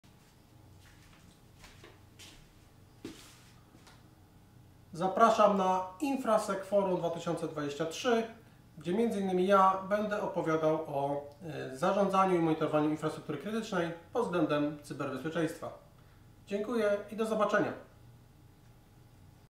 Zaproszenie na InfraSEC Forum 2023 – Nomios Poland & Fortinet
Zapraszamy do obejrzenia zapowiedzi wystąpienia Piotra Łyczby, Network Expert w Nomios Poland, na konferencji InfraSEC Forum 2023 - jedynego w swoim rodzaju wydarzenia poświęconego ochronie twardej infrastruktury.